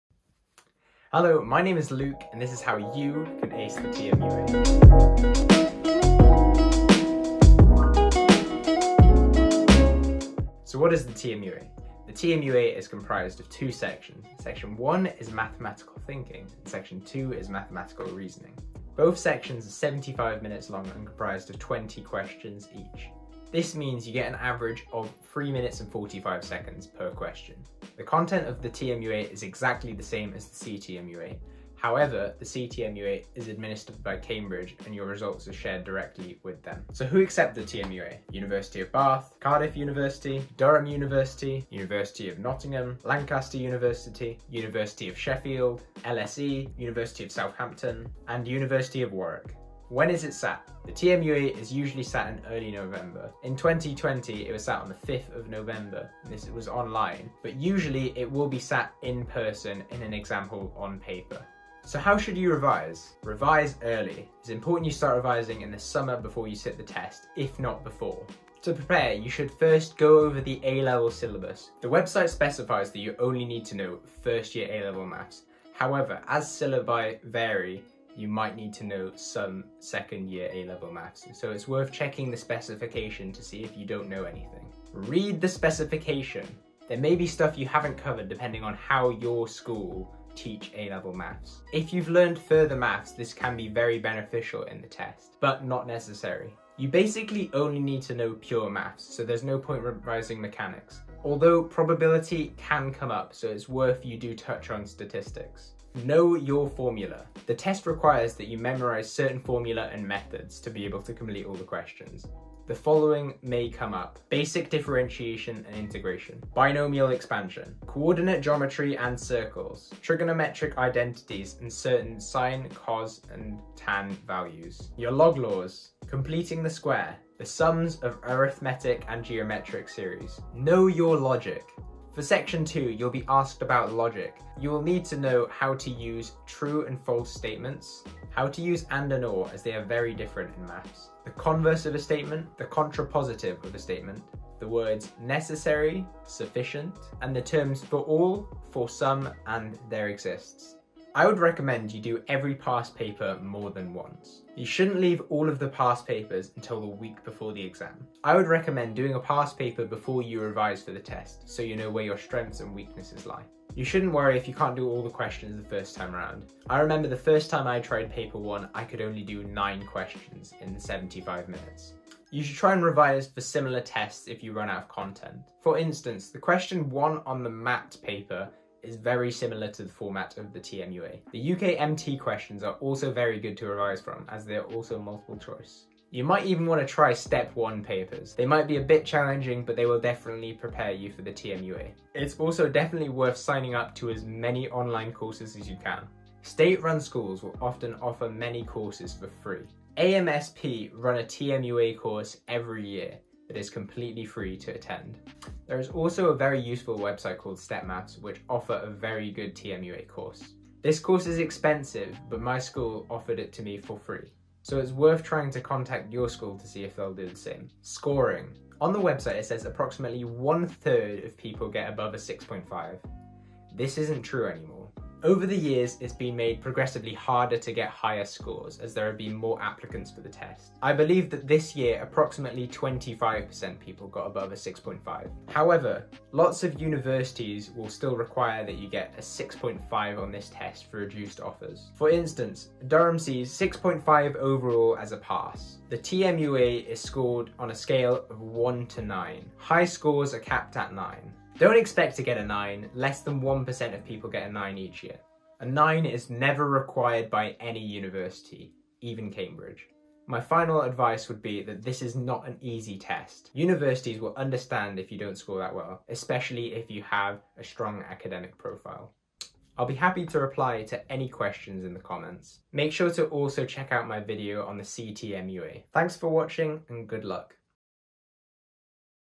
How to Ace the TMUA
This video is a brief description of the TMUA and some general tips and knowledge about the test.

The TMUA is the abbreviation of Test of Mathematics for University Admission.